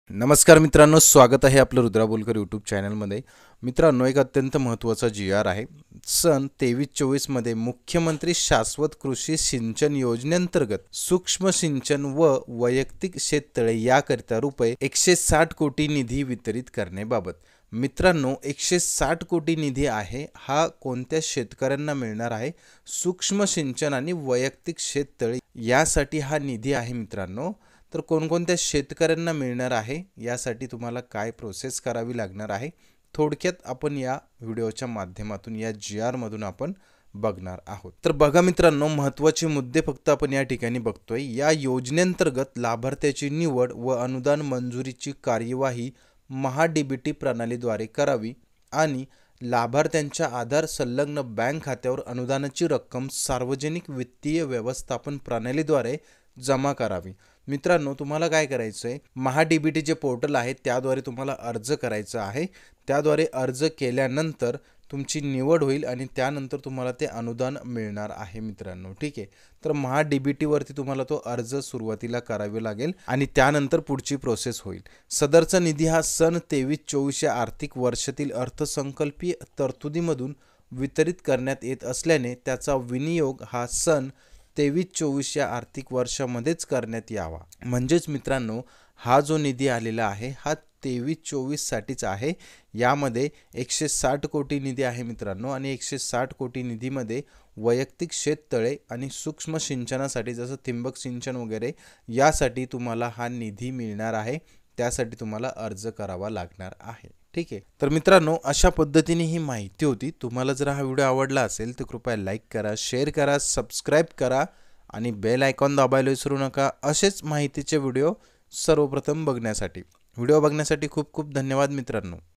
वैयक्तिक शेततळे व सूक्ष्म सिंचन साठी निधी मंजूर अर्ज कुठे करायचा
नमस्कार शेतकरी बांधवांनो वैयक्तिक शेततळे व सूक्ष्म सिंचन साठी निधी मंजूर अर्ज कुठे करायचा याविषयी या व्हिडीओ  मध्ये सविस्तर माहिती दिलेली आहे.

ठिंबक सिंचन व तुषार सिंचन साठी मुख्यमंत्री शाश्वत कृषी सिंचन योजना आता मिळणार ८०% अनुदान
Mukhymantri Shasvat Krushi Sinchan Yojana 2023-24
Mukhymantri Shasvat Krushi Sinchan Yojana

Rudra Bolkar
RUDRA BOLKAR
rudra bolkar
RuDrA BoLkAr